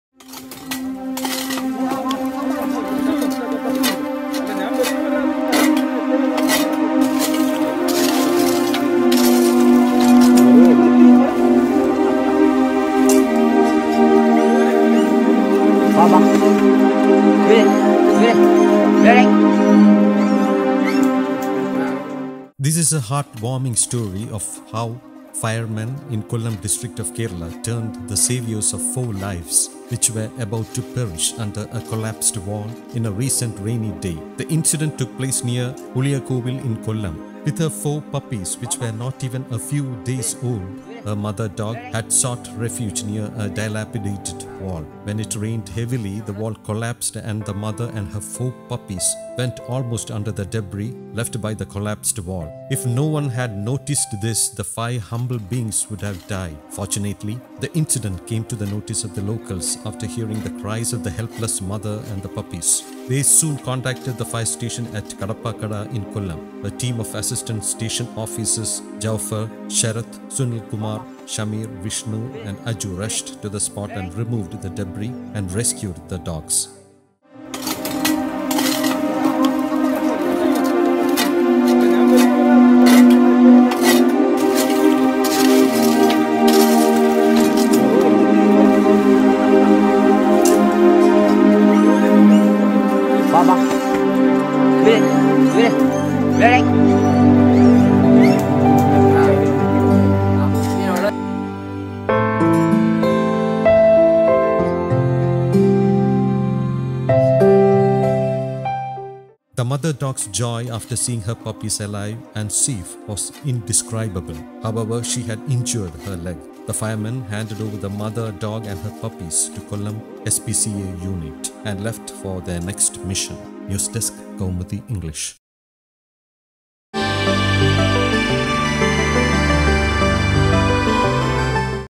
Five lives would have perished under collapsed wall but for these firefighter’s timely act!
This is a heart-warming story of how firemen in Kollam district of Kerala turned the saviours of four lives which were about to perish under a collapsed wall in a recent rainy day.

The incident took place near Uliyakovil in Kollam.
With her four babies which were not even a few days’ old, a mother dog had sought refuge near a dilapidated wall. When it rained heavily, the wall collapsed and the mother and her four puppies went almost under the debris left by the collapsed wall. If no one had noticed this, the five humble beings would have died.
Fortunately, the incident came to the notice of the locals after hearing the cries of the helpless mother and the puppies. They soon contacted the fire station at Kadapakkada in Kollam. A team of Assistant Station Officers Jaufar, Sarath, Sunilkumar, Shamir, Vishnu and Aju rushed to the spot and removed the debris and rescued the dogs.

The mother dog’s joy after seeing her puppies alive and safe was indescribable. However, she had injured her leg. The firemen handed over the mother dog and her puppies to Kollam SPCA unit and left for their next mission.